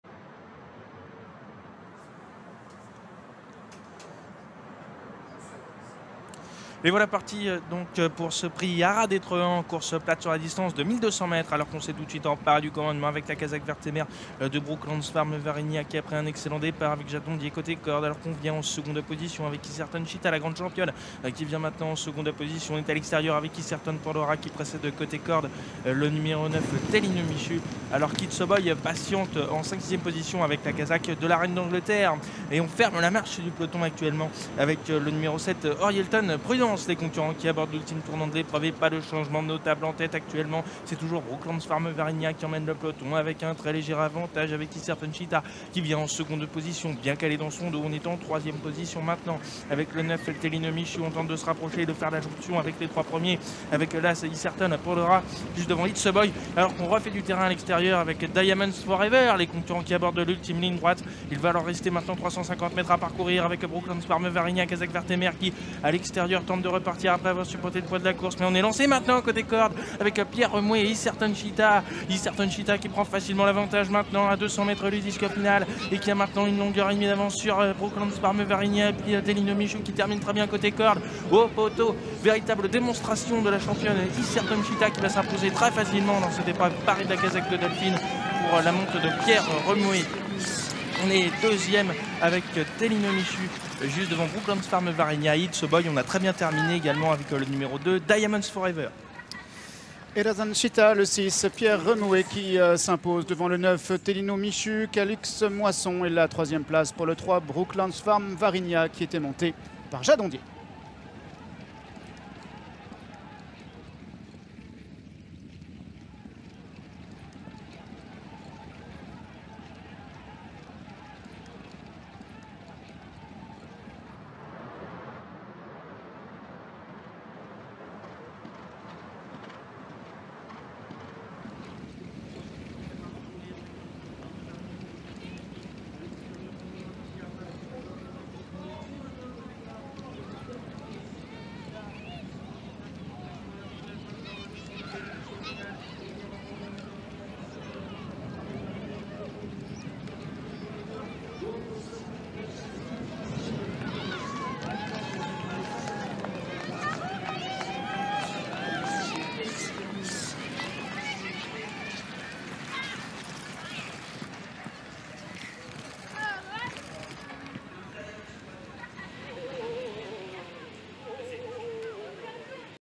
Championnat France Sire Poneys 2022: Prix du Haras d'Etreham (Poneys C Elite)
Grâce à France Galop, revivez la victoire de Pierre Remoué et Heatherton Cheetah dans le Prix du Haras d'Etreham (Poneys C Elite) disputé jeudi 04 août 2022, premier jour du 6e Championnat de la Fédération Française d'Equitation de courses plates à poneys / Championnats de France Sire Poneys.